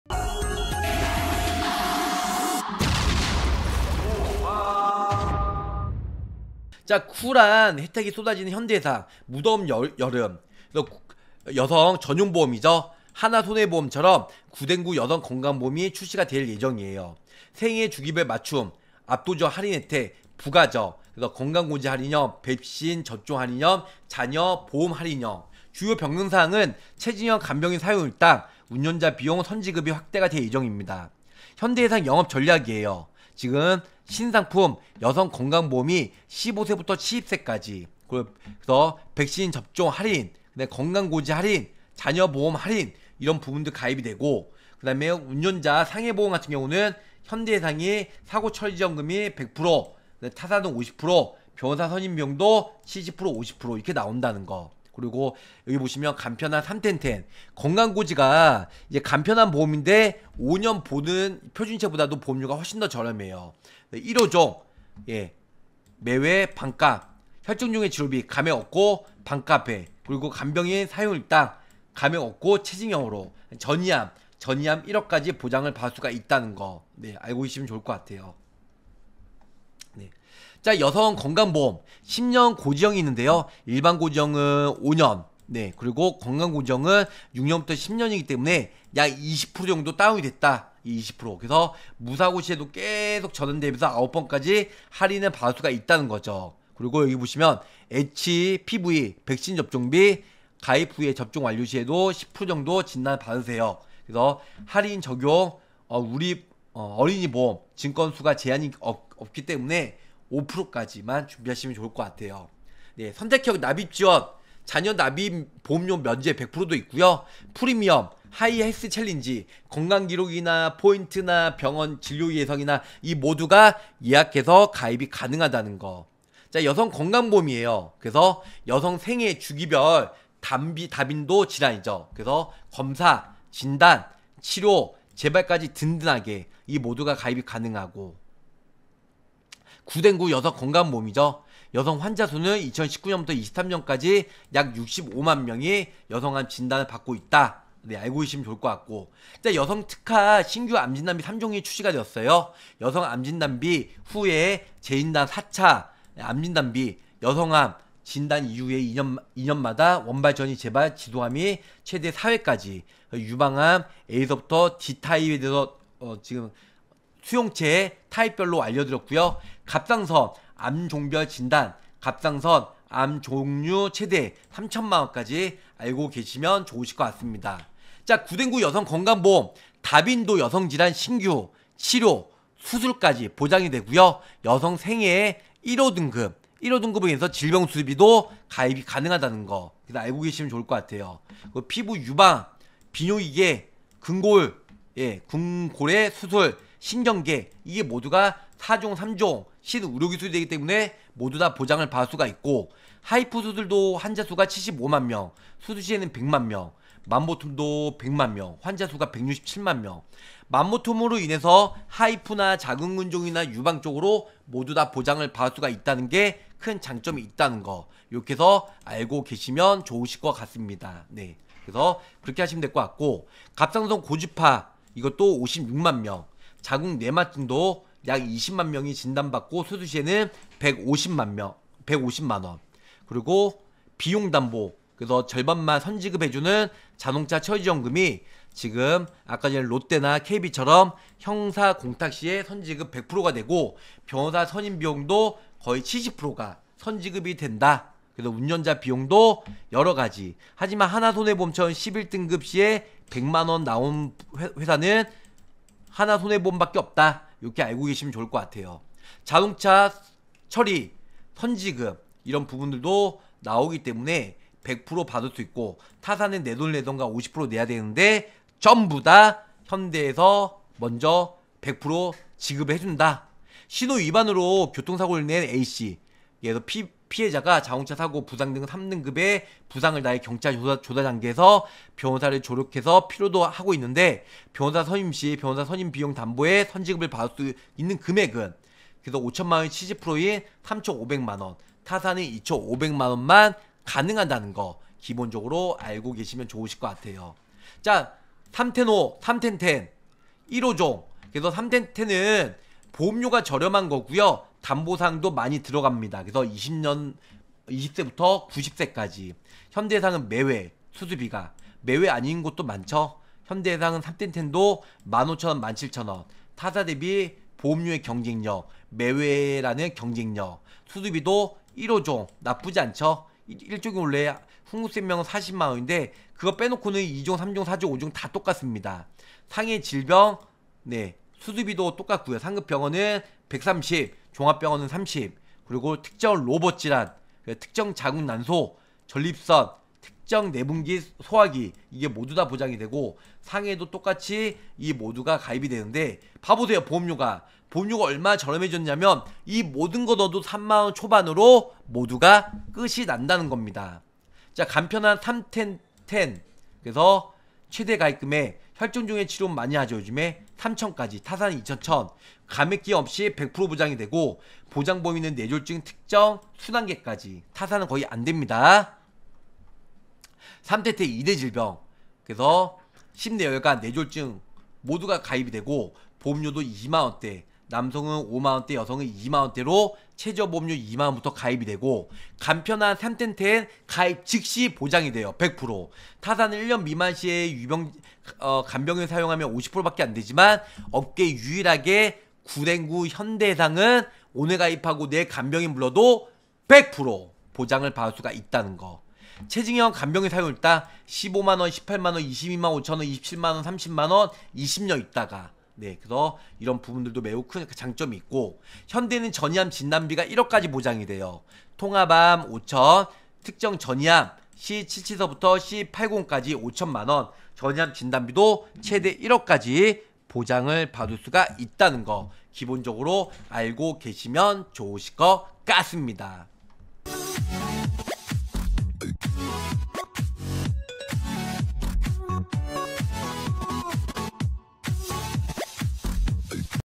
8월 현대해상 신상품이슈안내!! (굿앤굿여성전용보험출시~)
안녕하세요~~

현대해상 8월 신상품 이슈안내올려드립니다
여성 전용보험 출시예정이니 비교를 통해
준비하세요~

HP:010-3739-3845
카톡친구추가:r2an
카톡플러스친구:보험왕검색